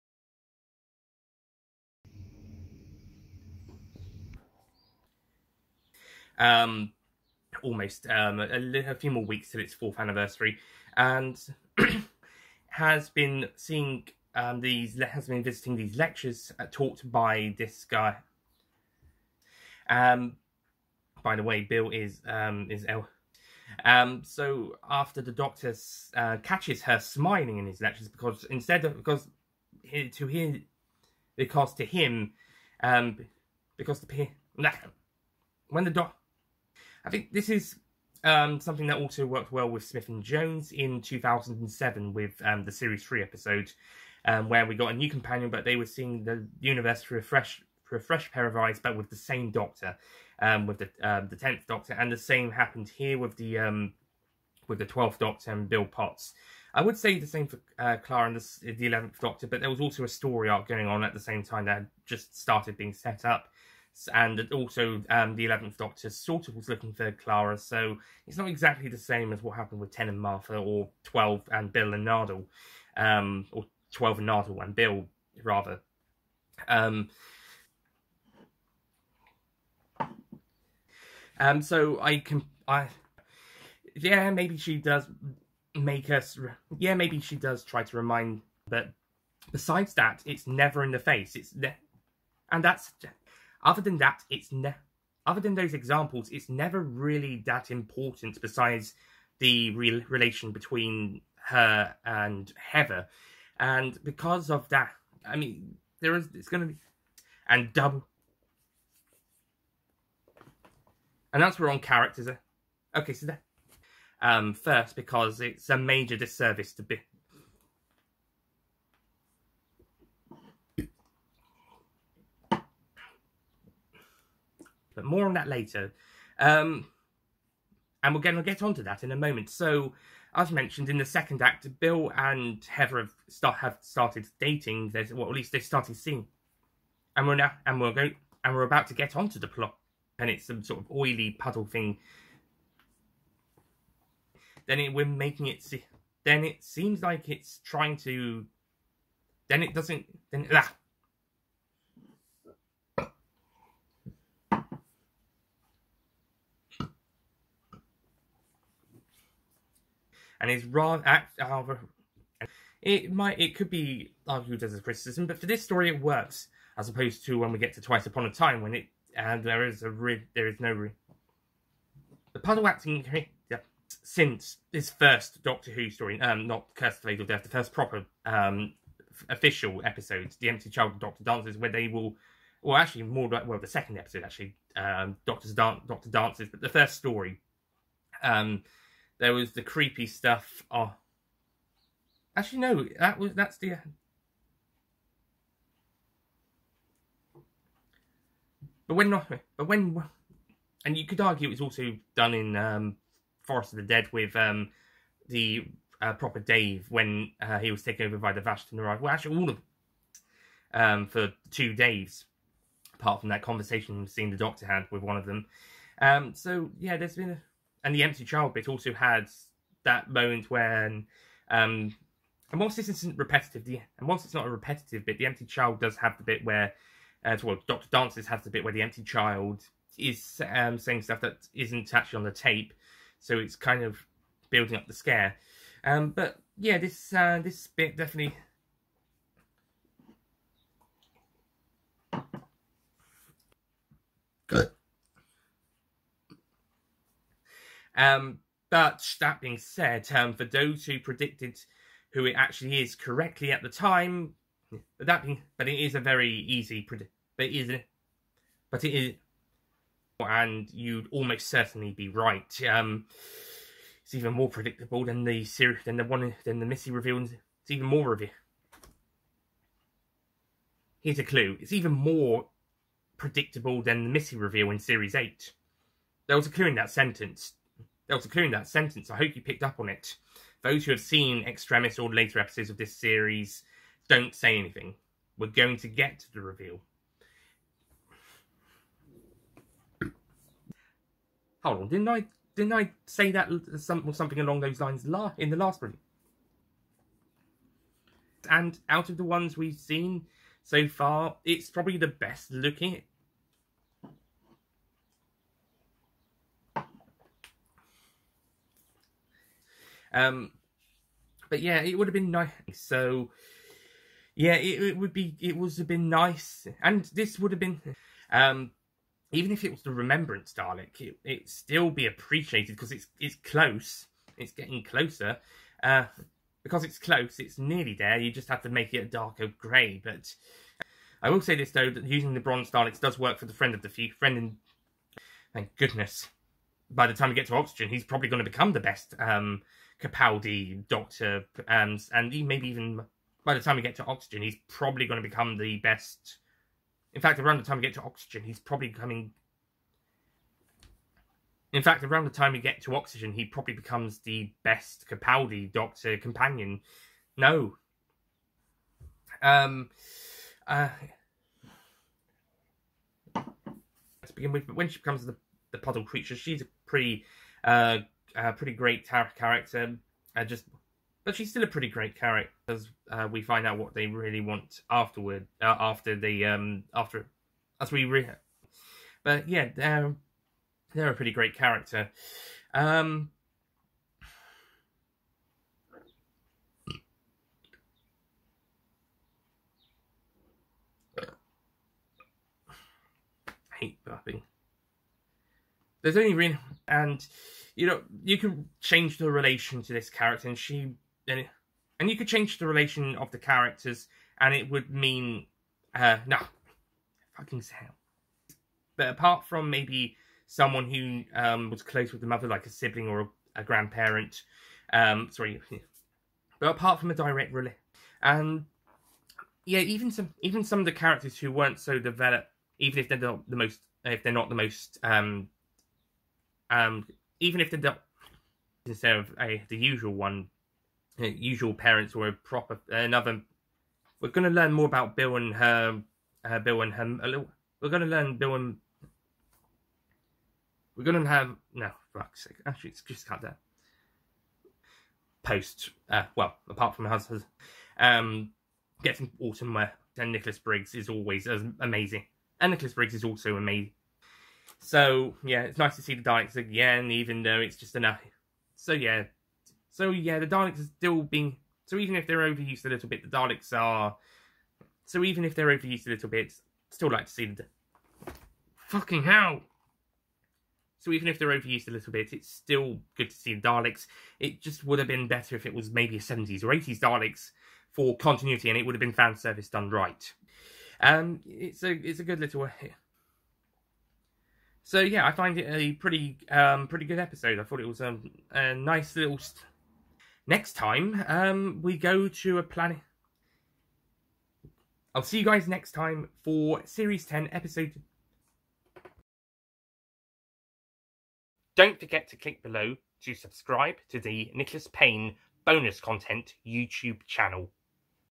Doctor Who: The Pilot Revisitation Review Bloopers
The Bloopers to my 2021 revisitation review of "Doctor Who: The Pilot".

2021.